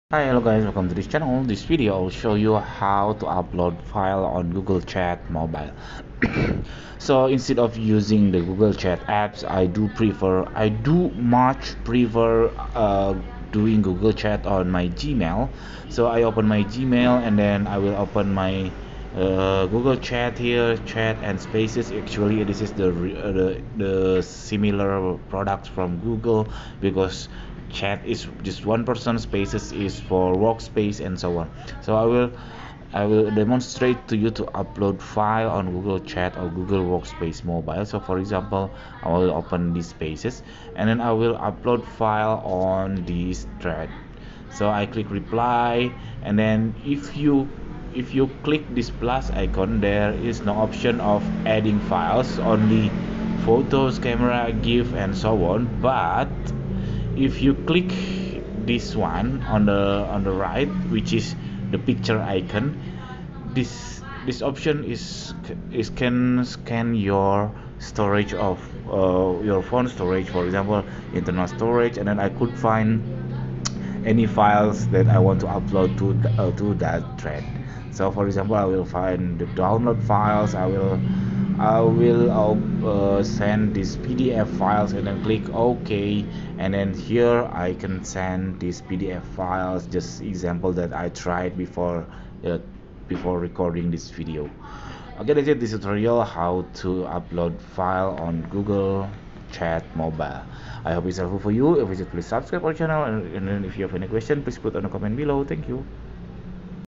How to Upload Files on Google Chat / Google Spaces on Mobile
easy to do how to.upload files on Google Chat, just follow these steps on the video..